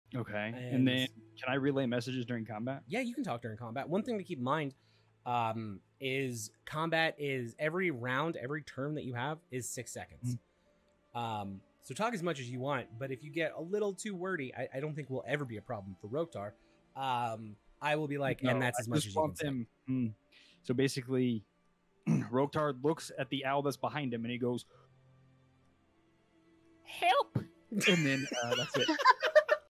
help
Sif tries to learn about communicating during combat.

Come check me out Wednesday - Friday at 7:30pm PST! And every other Saturday for Adventures in Solatia at 3:30pm PST!

Featuring fellow streamers!